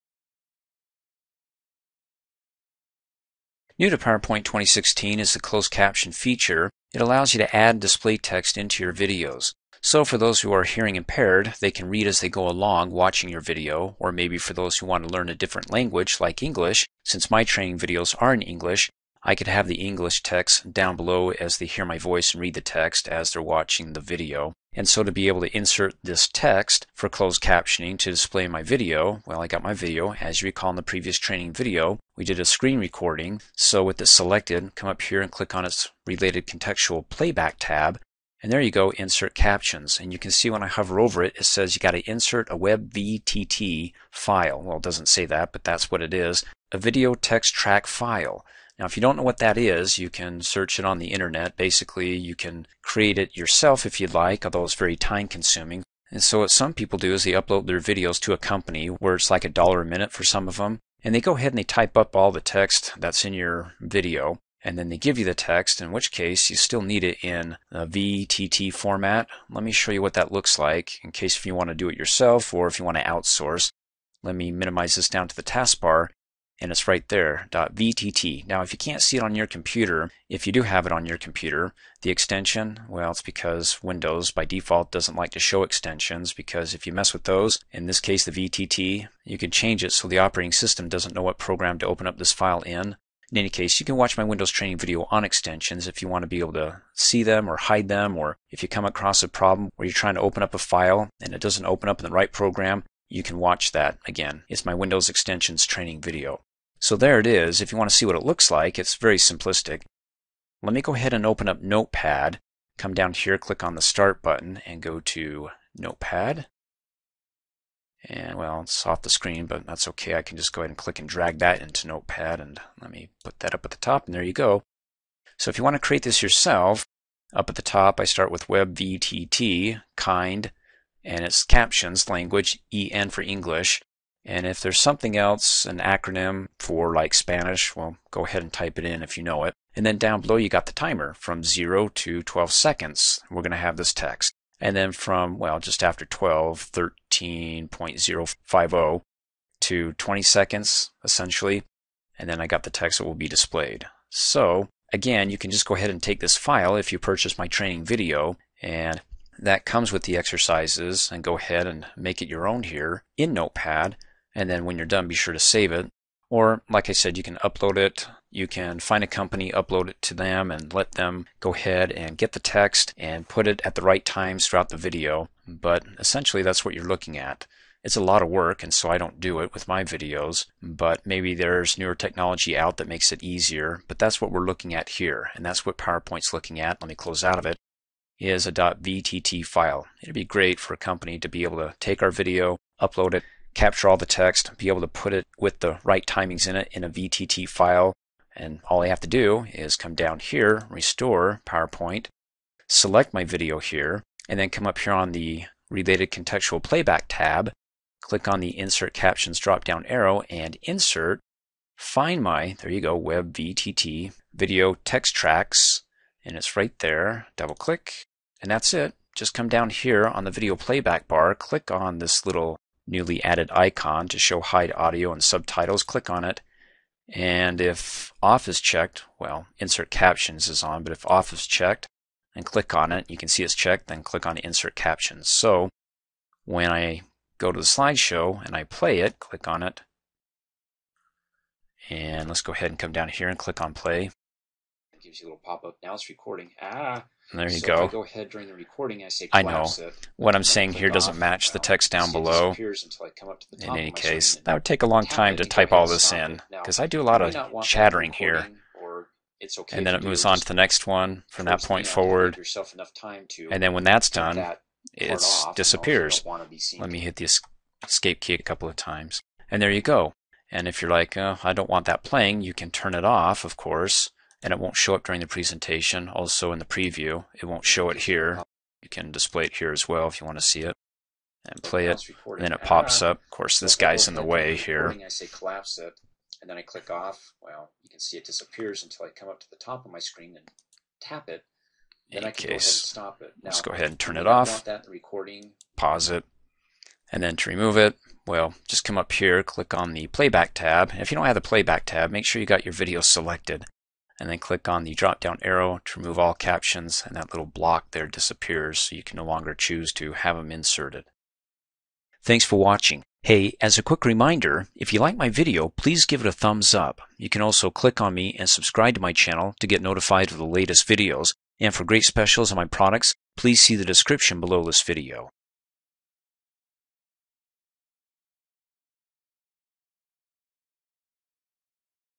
Microsoft PowerPoint 2016: Closed Caption
Microsoft PowerPoint 2016 training video on how to add Closed Caption to your PowerPoint presentation slide show; which allow you to add and display text in your videos.